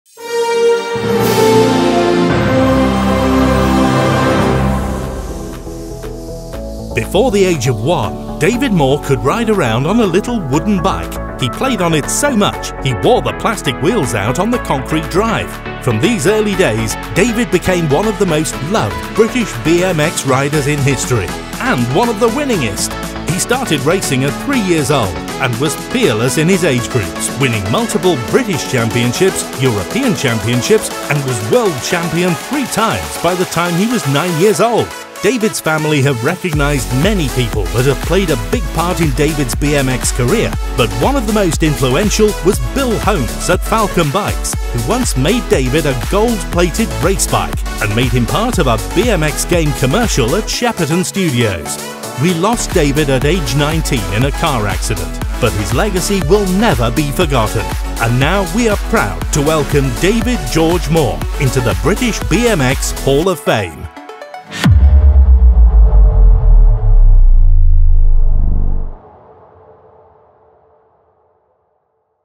BBMXHOF Inductees 2022: DAVID MAW
British BMX Hall of Fame Class of 2022: Lifetime Achievement Award, David Maw
Before the age of one, David Moore could ride around on a little wooden bike, he played on it so much he wore the plastic Wheels out on the concrete drive.

From these early days David became one of the most loved British BMX riders in history and one of the winningest. He started racing at 3 years old and was Fearless in his age groups winning multiple British Championships, European championships and was world champion three times by the time he was 9 years old.

David's family have recognized many people that have played a big part in David's BMX career but one of the most influential was Bill Holmes at Falcon bikes who once made David a goldplated race bike and made him part of a BMX game commercial at shepperton Studios. We lost David at age 19 in a car accident but his legacy will never be forgotten and now we are proud to welcome David George Moore into the British BMX Hall of Fame.